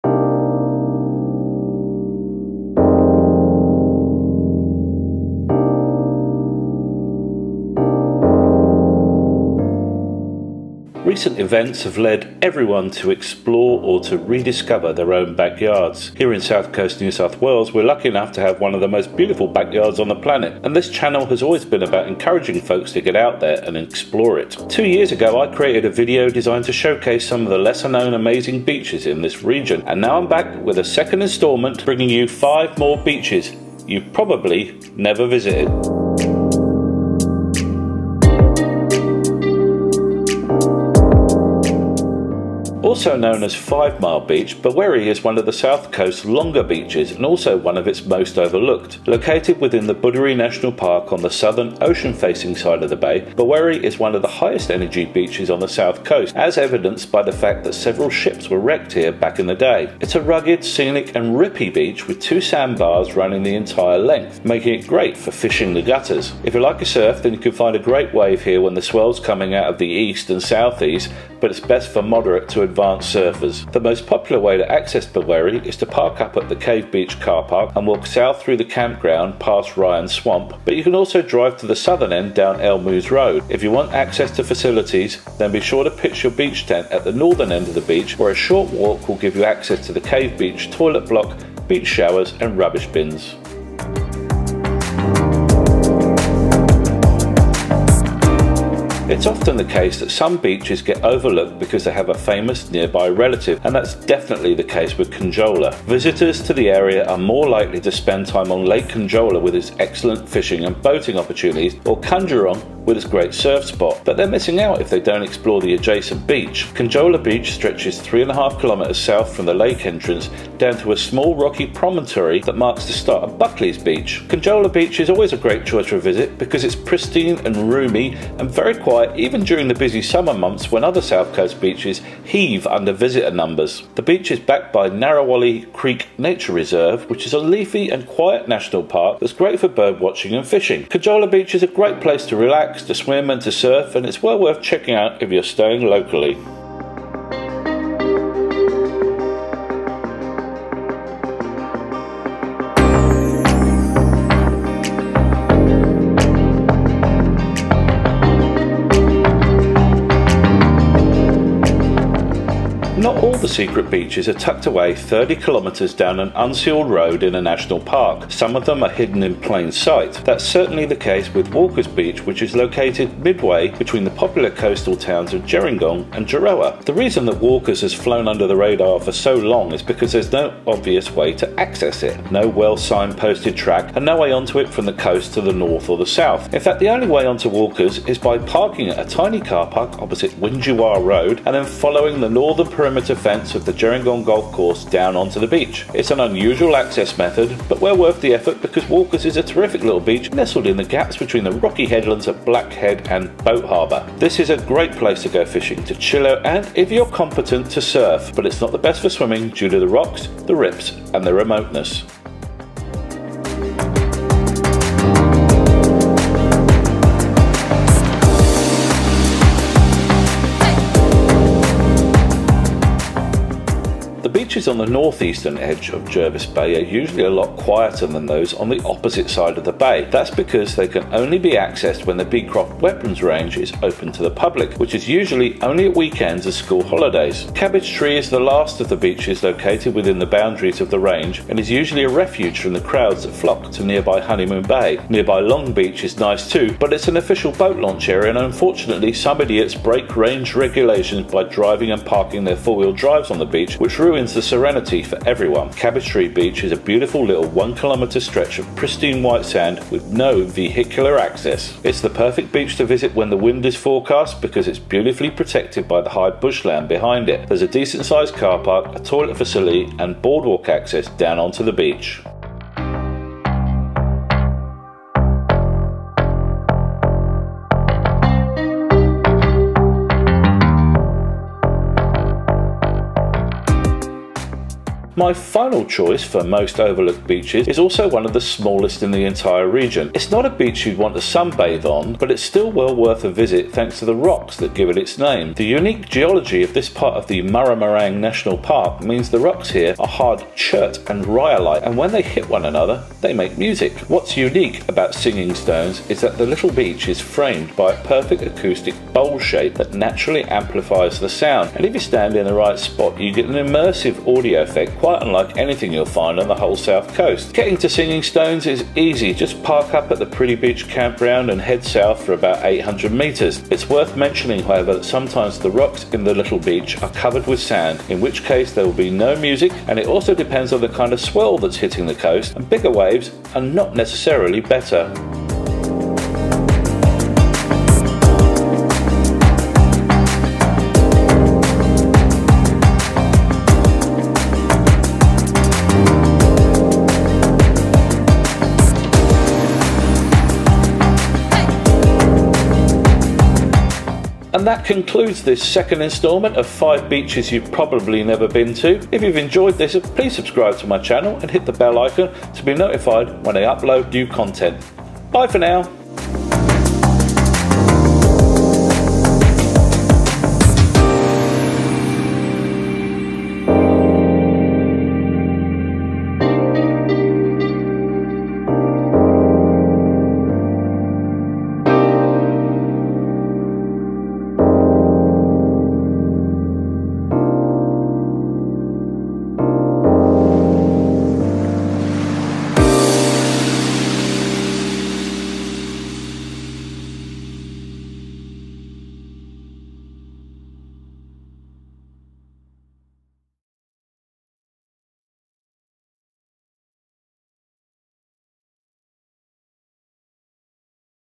Five of the Best Beaches in South Coast New South Wales You've (Probably) Never Been To (Part Two)
Welcome to another special edition.  In this episode I showcase five more amazing beaches that most people have never been to before, including the amazing Singing Stones Beach and Walkers Beach near Gerringong. So please settle in, lean back and enjoy the scenery as we explore the beauty of east coast Australia.

Those beaches:
0:41 Bherwerre (Five Mile) Beach
1:38 Conjola Beach
2:42 Walkers Beach
3:43 Cabbage Tree Beach
4:41 Singing Stones Beach

FILMING GEAR:
Canon 7D Mark II
Fujifilm X-T4
PolarPro Fifty/Fifty Dome Port
iPhone XR